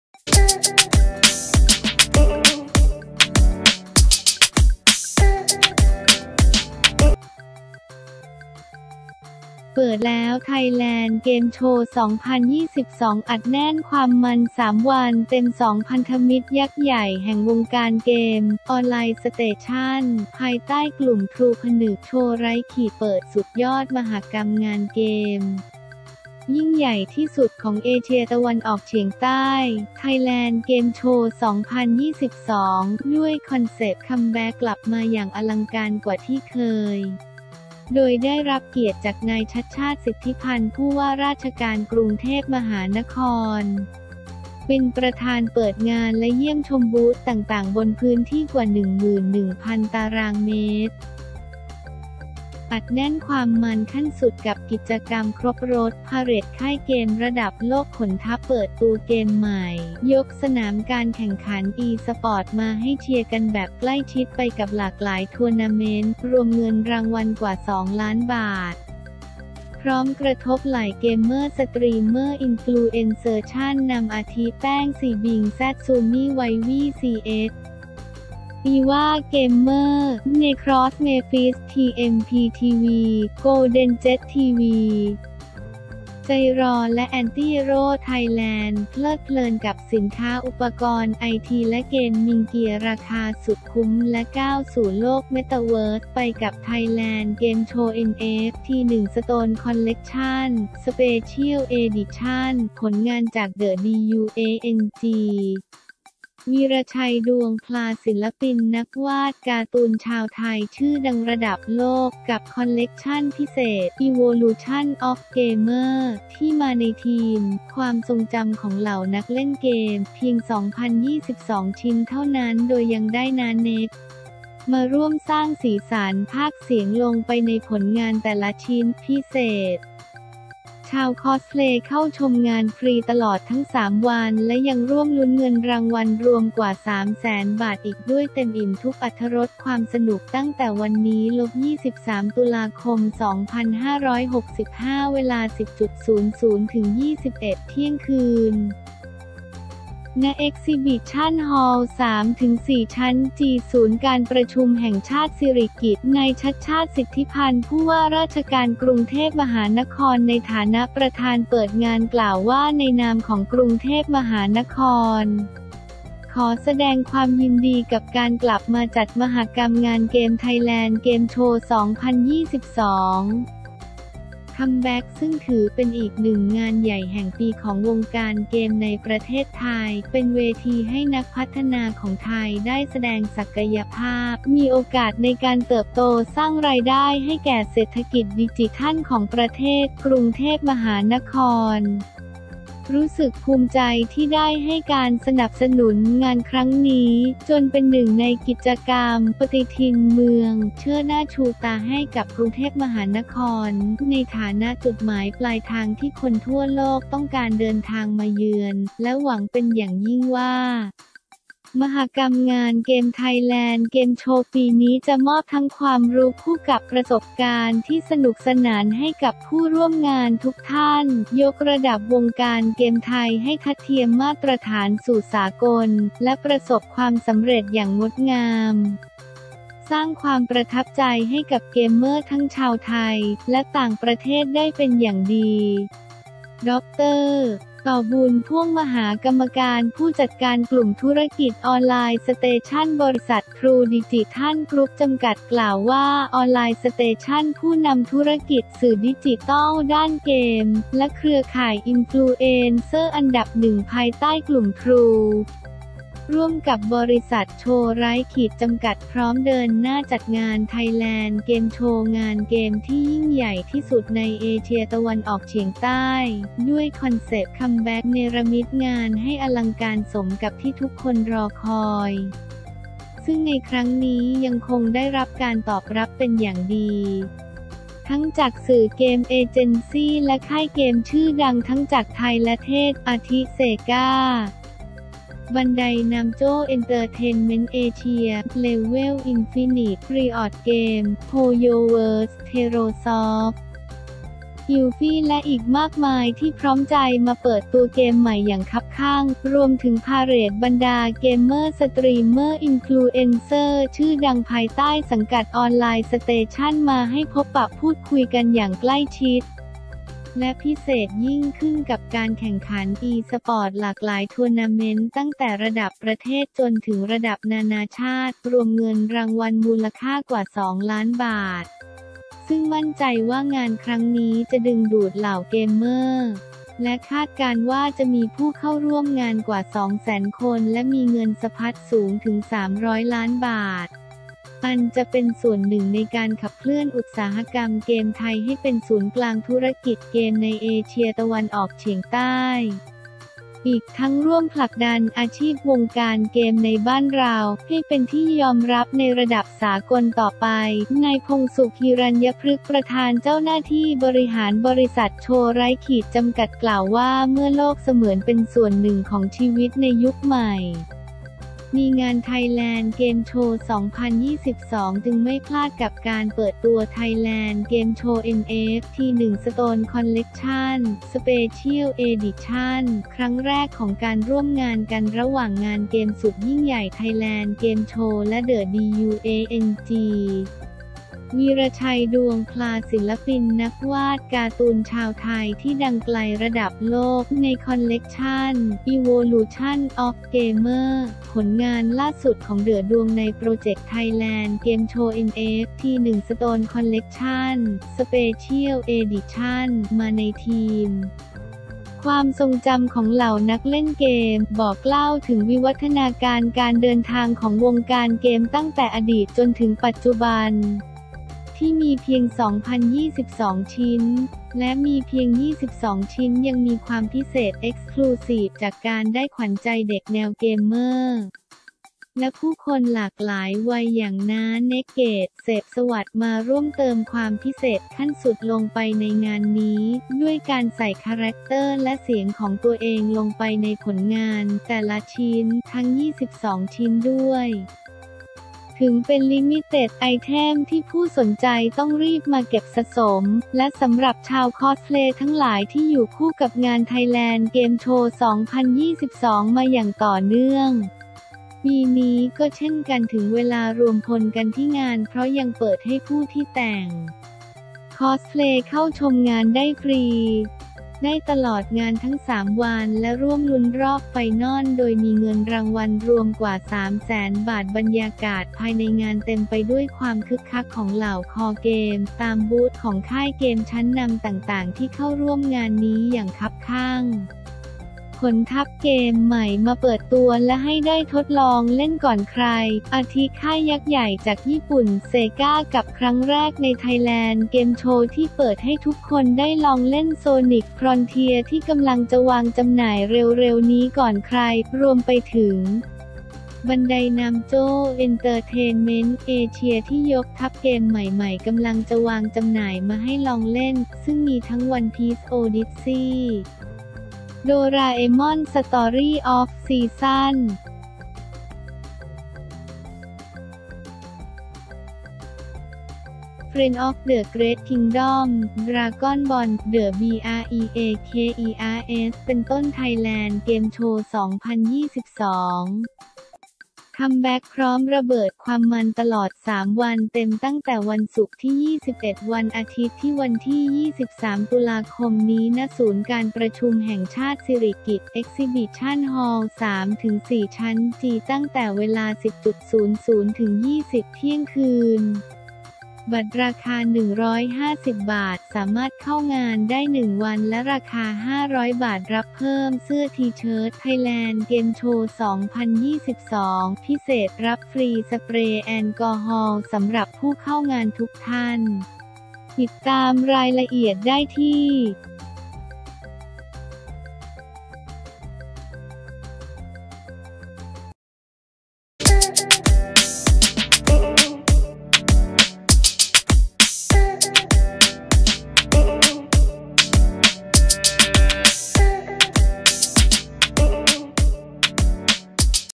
เปิดแล้ว Thailand Game Show 2022 อัดแน่นความมันส์ 3 วันเต็ม
2 พันธมิตรยักษ์ใหญ่แห่งวงการเกม “ออนไลน์ สเตชั่น” ภายใต้กลุ่มทรู ผนึก “โชว์ไร้ขีด” เปิดสุดยอดมหกรรมงานเกม ยิ่งใหญ่ที่สุดของเอเชียตะวันออกเฉียงใต้ “Thailand Game Show 2022” ด้วยคอนเซ็ปต์ “Come Back” กลับมาอย่างอลังการกว่าที่เคย โดยได้รับเกียรติจาก นายชัชชาติ สิทธิพันธุ์ ผู้ว่าราชการกรุงเทพมหานคร เป็นประธานเปิดงานและเยี่ยมชมบูธต่างๆ บนพื้นที่กว่า 11,000 ตรม. อัดแน่นความมันส์ขั้นสุดกับกิจกรรมครบรส พาเหรดค่ายเกมระดับโลกขนทัพเปิดตัวเกมใหม่ ยกสนามการแข่งขัน E-Sport มาให้เชียร์กันแบบใกล้ชิดไปกับหลากหลายทัวร์นาเมนท์ รวมเงินรางวัลกว่า 2 ล้านบาท พร้อมกระทบไหล่เกมเมอร์ สตรีมเมอร์ อินฟลูเอนเซอร์ชั้นนำ อาทิ แป้ง zbing z SoomnyVivii Ch, Eva Gamer, Necross Melphist, TMP TV, GoldenJet TV, JAI RAW และ Antihero Thailand เพลิดเพลินกับสินค้า อุปกรณ์ไอที และเกมมิงเกียร์ราคาสุดคุ้ม และก้าวสู่โลก Metaverse ไปกับ Thailand Game Show NFT 1st Collection (Special Edition) ผลงานจาก The DUANG – วีระชัย ดวงพลา ศิลปินนักวาดการ์ตูนชาวไทยชื่อดังระดับโลก กับคอลเลกชันพิเศษ “Evolution of Gamers” ที่มาในธีม “ความทรงจำของเหล่านักเล่นเกม” เพียง 2,022 ชิ้นเท่านั้น โดยยังได้ น้าเน็ก มาร่วมสร้างสีสันพากย์เสียงลงไปในผลงานแต่ละชิ้น พิเศษ! ชาว Cosplay เข้าชมงานฟรีตลอด ทั้ง 3 วัน และยังร่วมลุ้นเงินรางวัลรวมกว่า 300,000 บาทอีกด้วย เต็มอิ่มทุกอรรถรสความสนุกตั้งแต่วันนี้ – 23 ตุลาคม 2565 เวลา 10.00 – 21.00 น. ณ Exhibition Hall 3 – 4 ชั้น G ศูนย์การประชุมแห่งชาติสิริกิติ์
นายชัชชาติ สิทธิพันธุ์ ผู้ว่าราชการกรุงเทพมหานคร ในฐานะประธานเปิดงาน กล่าวว่า ในนามของกรุงเทพมหานคร ขอแสดงความยินดีกับการกลับมาจัดมหกรรมงานเกม Thailand Game Show 2022 : Come Back ซึ่งถือเป็นอีกหนึ่งงานใหญ่แห่งปีของวงการเกมในประเทศไทย เป็นเวทีให้นักพัฒนาของไทยได้แสดงศักยภาพ มีโอกาสในการเติบโต สร้างรายได้ให้แก่เศรษฐกิจดิจิทัลของประเทศ กรุงเทพมหานคร รู้สึกภูมิใจที่ได้ให้การสนับสนุนงานครั้งนี้ จนเป็นหนึ่งในกิจกรรม “ปฏิทินเมือง” เชิดหน้าชูตาให้กับกรุงเทพมหานคร ในฐานะจุดหมายปลายทางที่คนทั่วโลกต้องการเดินทางมาเยือน และหวังเป็นอย่างยิ่งว่า มหกรรมงานเกม Thailand Game Show ปีนี้ จะมอบทั้งความรู้คู่กับประสบการณ์ที่สนุกสนานให้กับผู้ร่วมงานทุกท่าน ยกระดับวงการเกมไทยให้ทัดเทียมมาตรฐานสู่สากล และประสบความสำเร็จอย่างงดงาม สร้างความประทับใจให้กับเกมเมอร์ทั้งชาวไทย และต่างประเทศได้เป็นอย่างดี